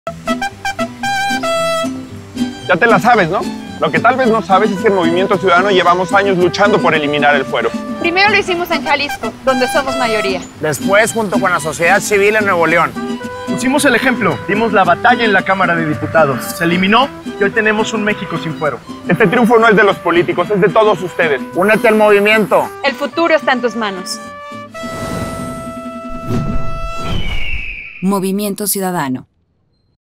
No permitiremos que el PRI frene en el Senado NoMásFuero - Movimiento Ciudadano
El PRI frenó la iniciativa contra el fuero en el Senado y no lo vamos a permitir.
Aunque insista en mantener este privilegio de sus políticos corruptos, nosotros ya logramos eliminarlo en Jalisco, Nuevo León, en la Cámara de Diputados y NO pararemos hasta tener un MéxicoSinFuero.